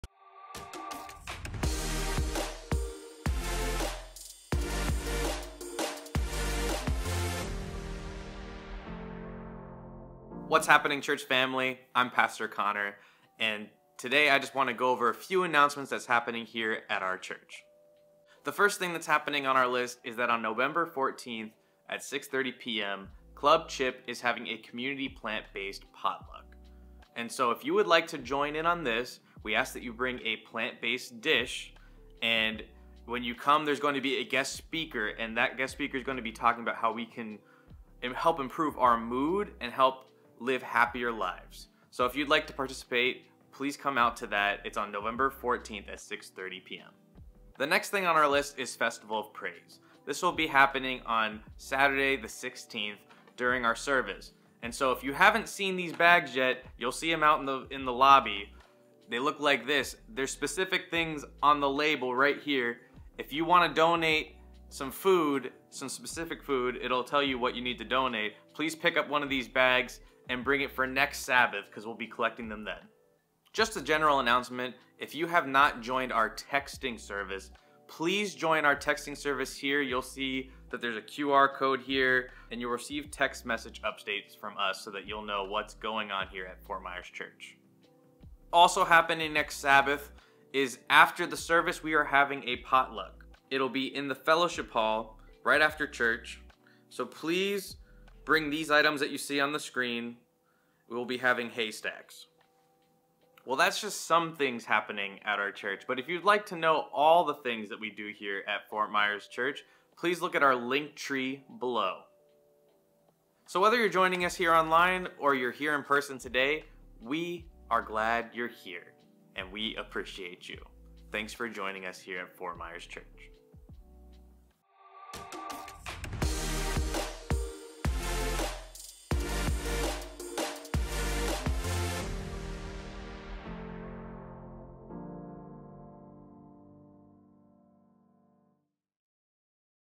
2024 Nov 9 - What's Happening at the Fort Myers SDA Church - Asso Pastor Connor Yonkers
2024 Nov 9 - What's Happening at the Fort Myers SDA Church - Asso Pastor Connor Yonkers